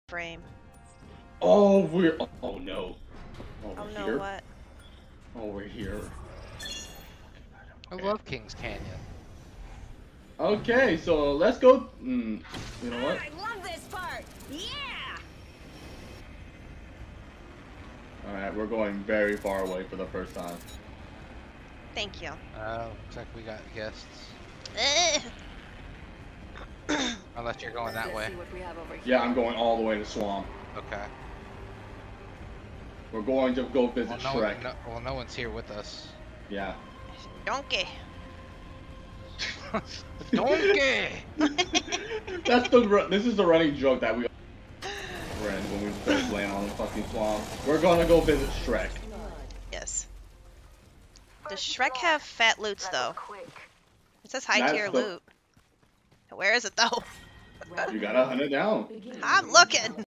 Donkey! We're going to visit Shrek Does Shrek have phat loots though
Stream Clip. Going to visit Shrek in his swamp and see if he has any fat loots